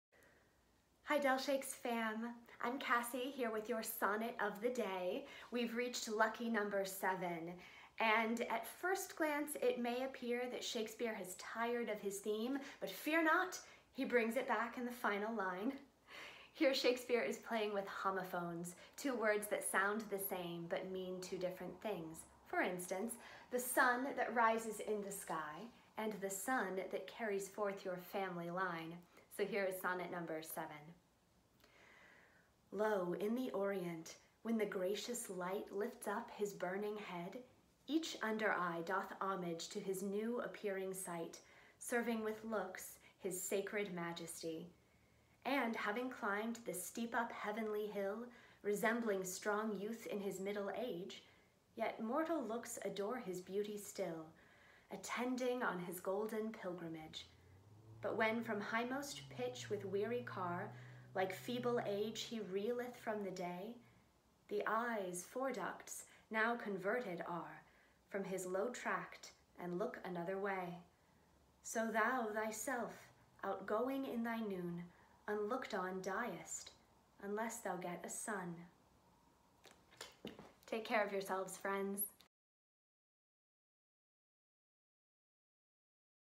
Sonnet 7
Del Shakes staff member Cassie Alexander has your sonnet of the day, getting us through our socially distant times with a little iambic pentameter. Recorded 3/27/20. See delshakes.org for more info.